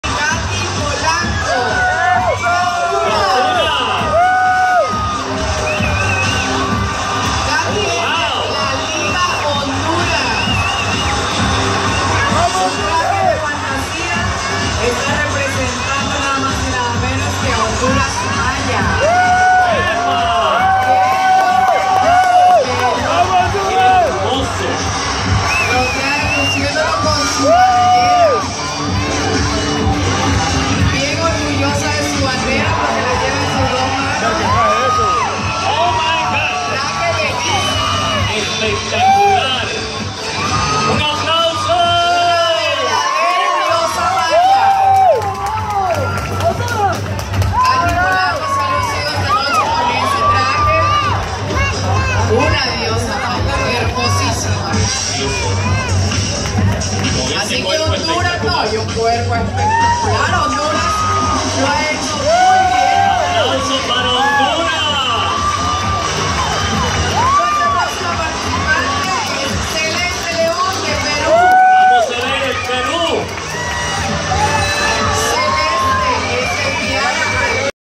Miss Gran Latina Usa / Traje Tipico - Kathy Polanco
Traje Tipico - Designer " Adolph Canaca " Gran Deseñador de L.A graciass mucho por su tiempo y mandarme su bello traje tipico igual a Martin Ortiz y Dustyn Martinez .